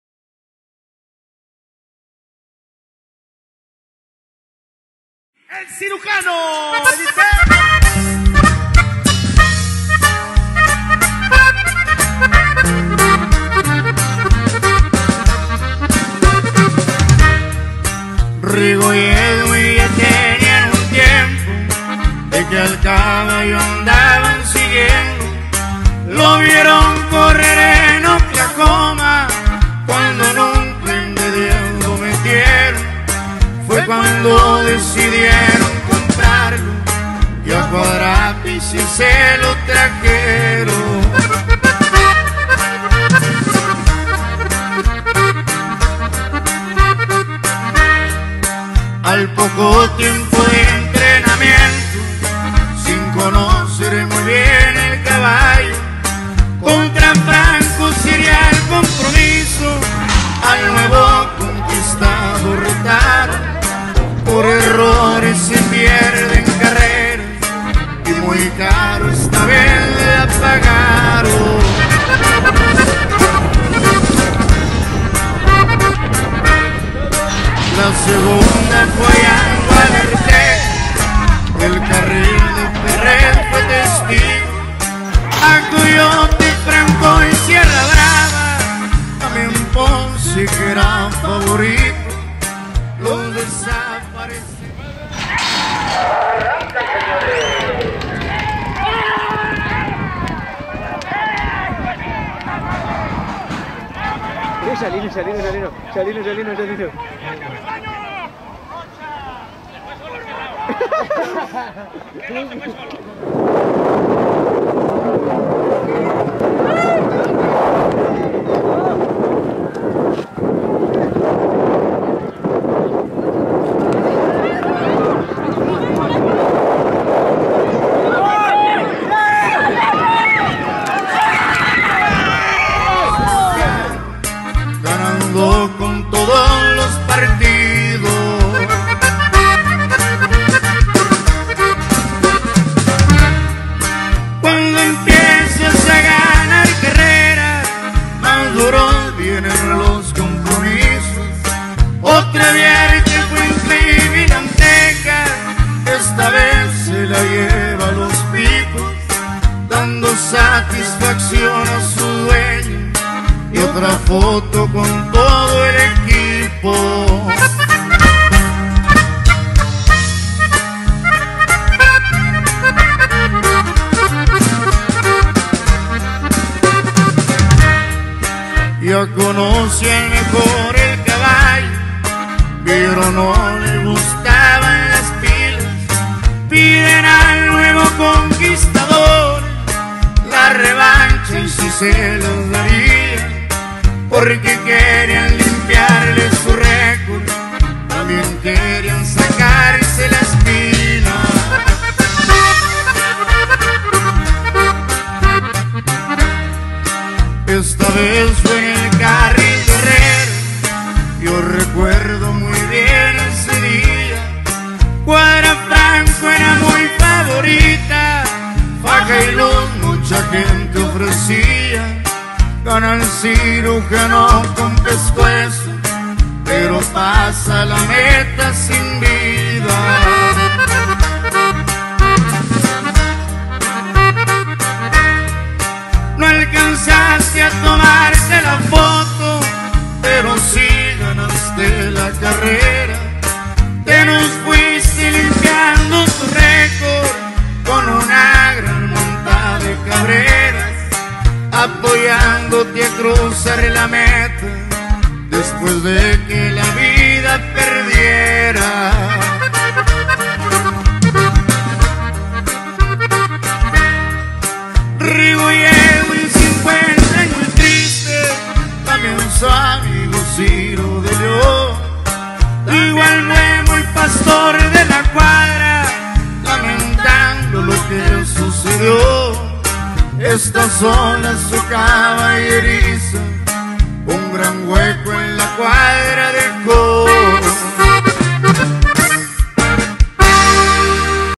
El Jamaiquino Cuadra Rocha vs El Misionero Cuadra Valenzuela
El Jamaiquino Cuadra Rocha vs El Misionero Cuadra Valenzuela en RAncho Los Alamos de Hesperia, California.